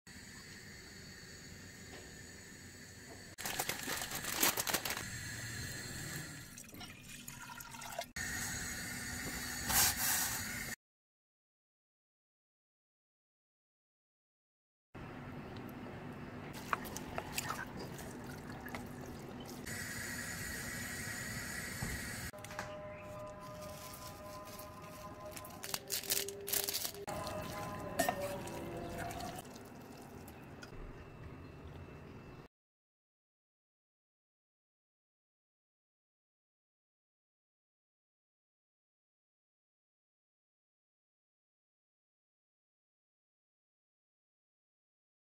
I tried The FIRE Ramen Challenge 🥵🥵🥵🥵🥵🥵🥵🥵🥵🥵🥵🥵🥵🥵🥵🥵🥵🥵🥵🥵🥵🥵🥵🥵🥵🥵🥵
The Fire Ramen Challenge
Korean Spicy Noodles
Spicy Noodle
Spicy Ramen Challenge
Nuclear Fire Challenge
I am going to try The Fire Ramen Challenge along with me!
I have no tolerance for spice and i am so excited and terrified to try this! I'm afraid something bad is going to happen! Tune in to see what happens  🥵
Checkout other Recipes on my channel:

If you liked this video, give it a 👍